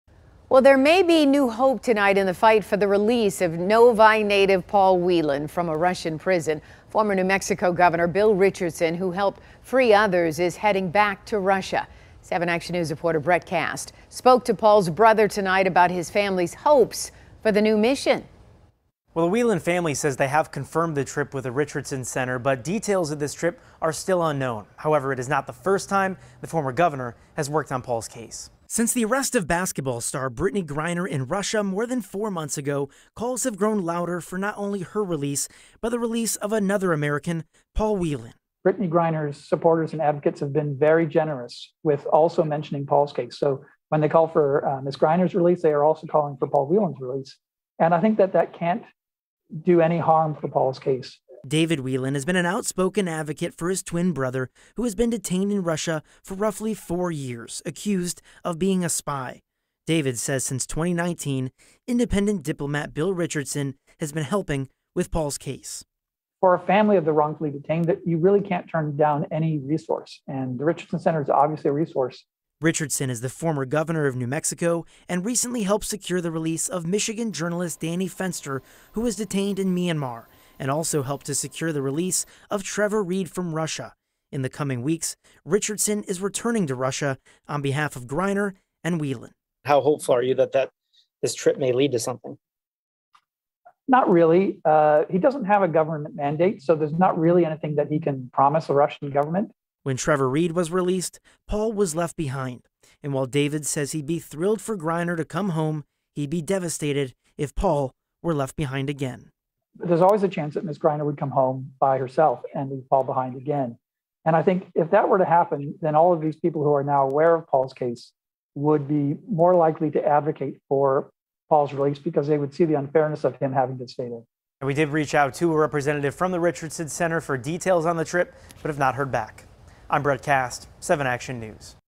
New hope for release of Paul Whelan
Bill Richardson, the former governor of New Mexico, is planning to head to Russia in the coming weeks in an effort to bring Paul Whelan and Brittney Griner back home.